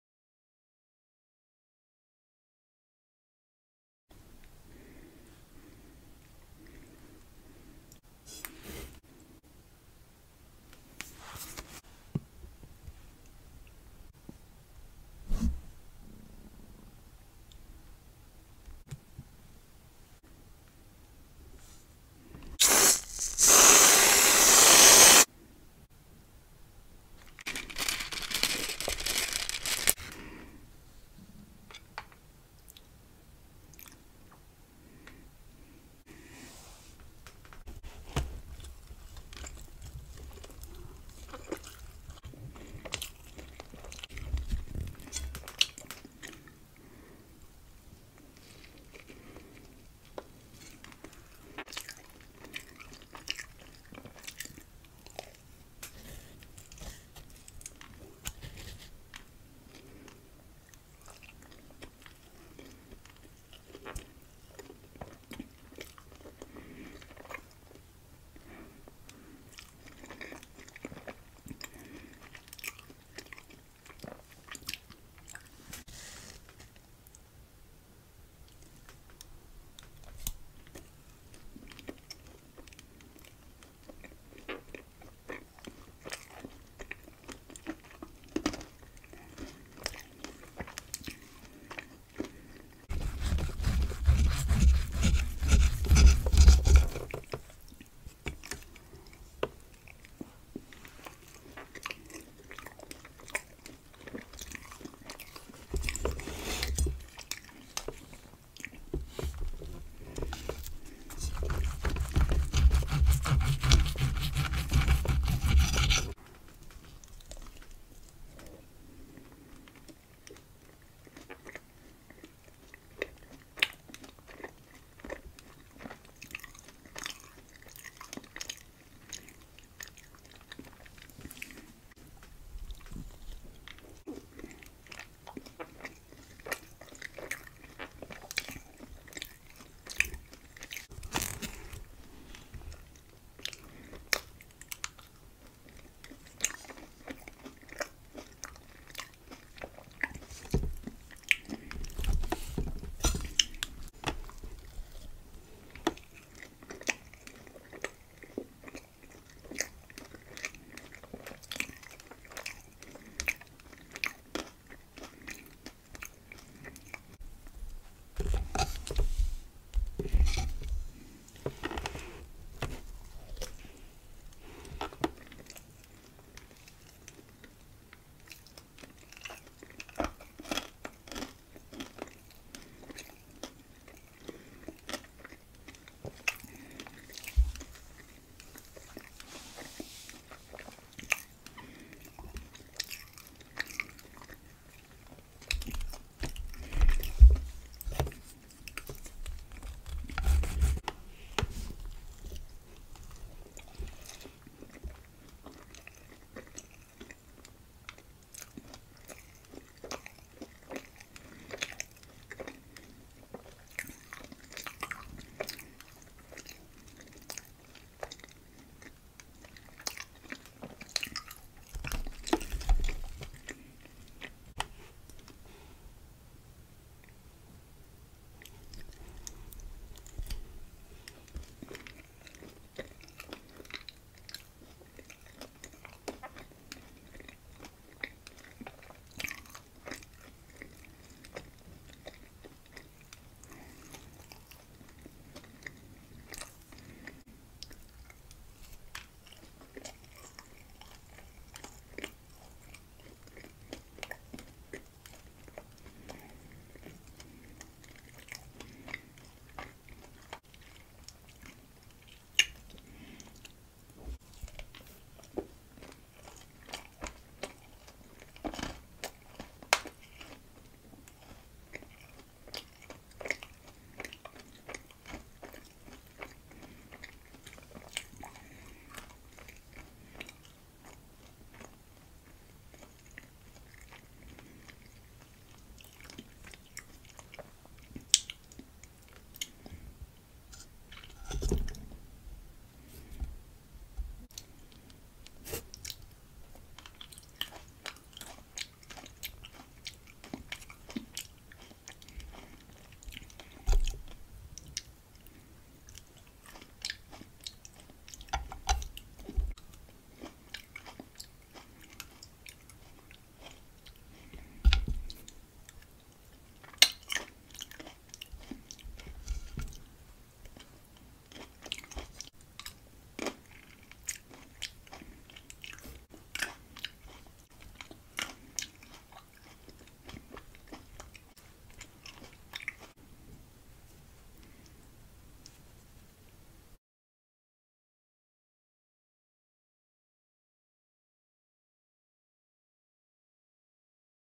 Blue Waffles ASMR ( No Talking )
Blue Waffles ASMR Eating ( No Talking ) @sweetLynnASMR   Blue Blue Berry waffles topped with butter , Captain Crunch Blue Syrup , whipped cream ,  blue Berries and, blue chocolates  yummy
💖🥰 If you enjoy please show some love with this link below 🥰💖